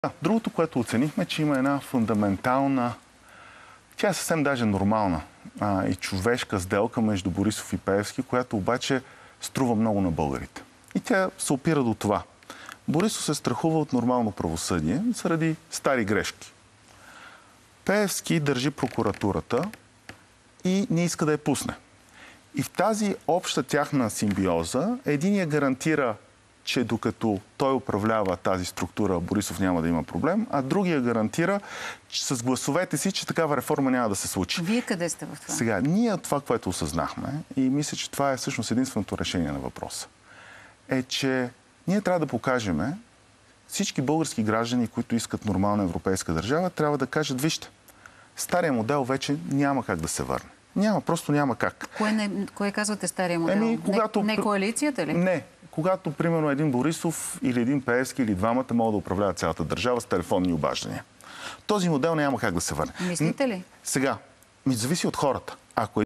Кирил Петков: Между Борисов и Пеевски има една сделка, която струва много на българите
❓ Коя е тази сделка?

Разберете от видеото: